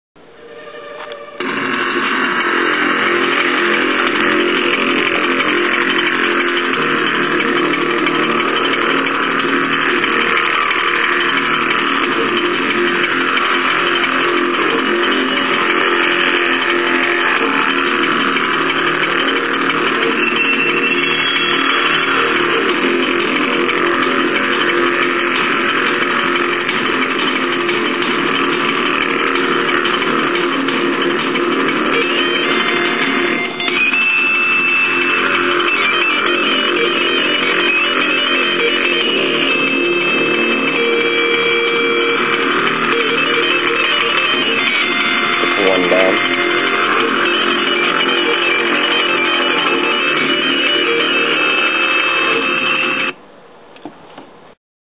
(Micropterus salmoides) wolfing down a feeder
Geraldine a 2inch largemouth  is in hot persuit of a feeder. he catches and engulfs it like a purebred monster